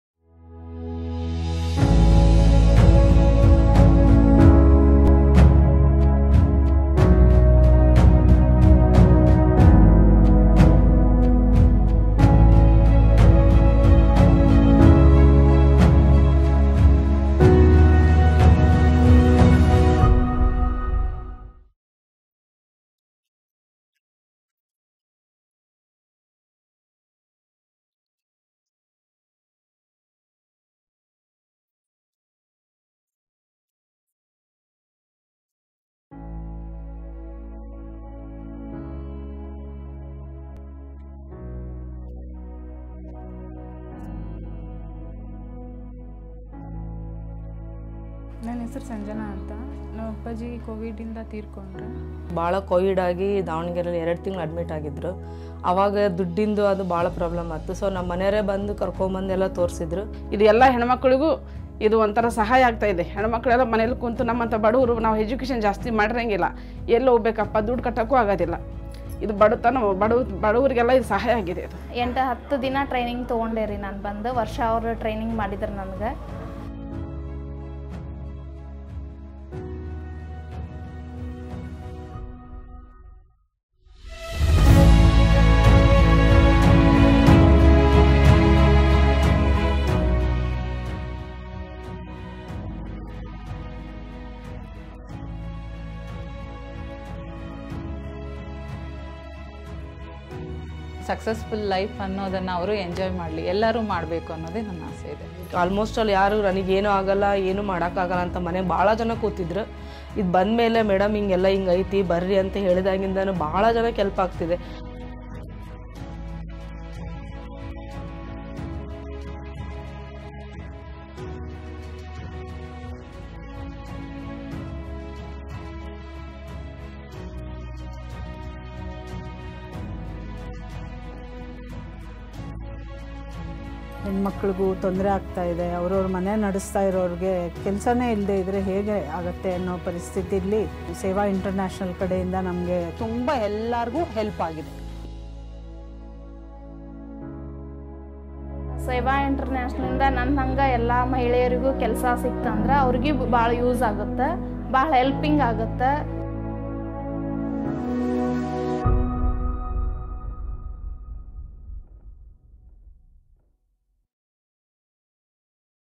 Sewa International SHE – CAF Project
SHE – CAF: Creation of Livelihood opportunities through skill development training for women from COVID affected families .
Sewa International is a Hindu faith-based 501(c)3 non-profit charity organization active in India, USA and other countries.